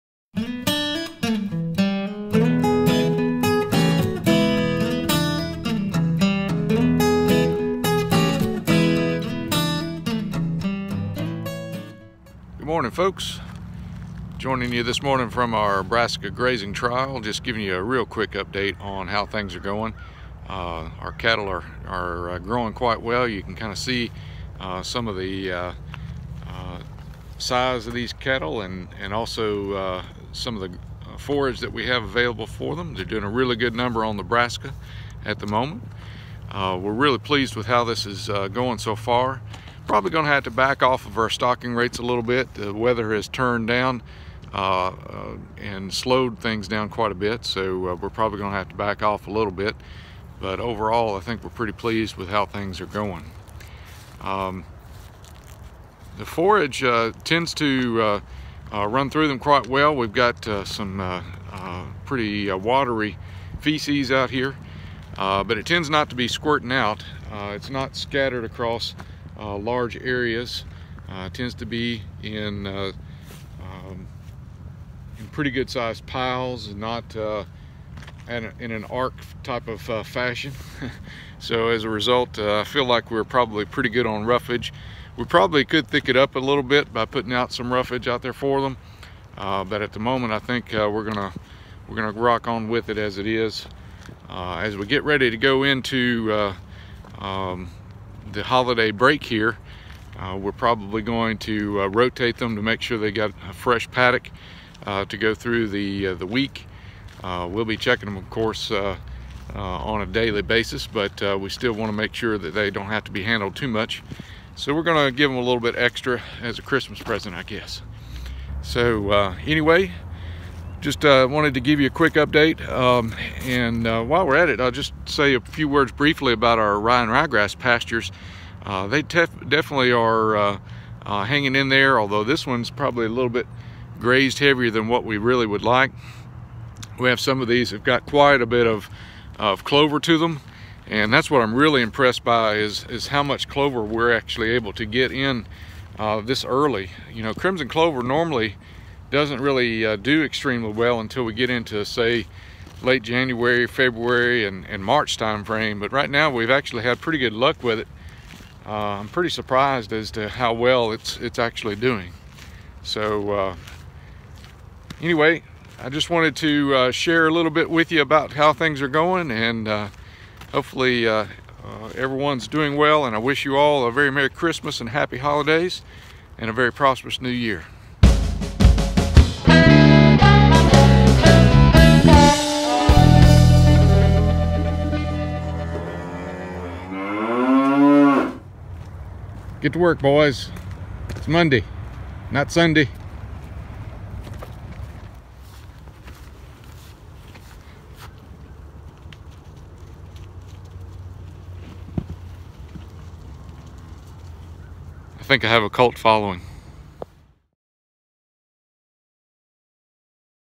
Grassman vlog 1710 Brassica Grazing Trial Update
In this episode, I give a quick update on the brassica grazing trial about a month into the trial.